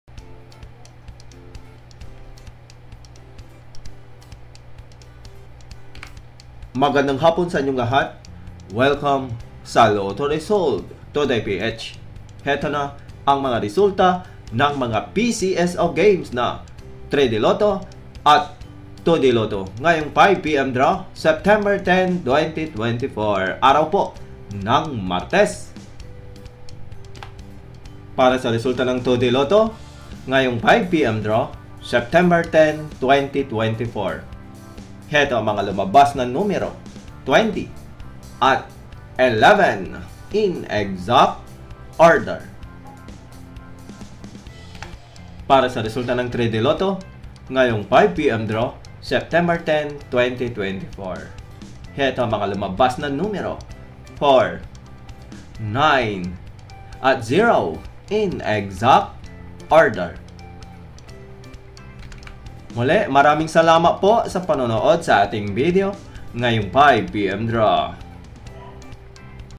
Lotto Result Today 5pm september 10, 2024 Swertres Ez2 Pcso
Lotto result today 5pm Draw September 10, 2024 (Tuesday) The 5pm afternoon draw pcso lotto results today are 3D Lotto (Swertres Result Today) and 2D Lotto (Ez2 Result Today).

0:00 Intro
0:27 2D Result 5pm
0:44 3D Result 5pm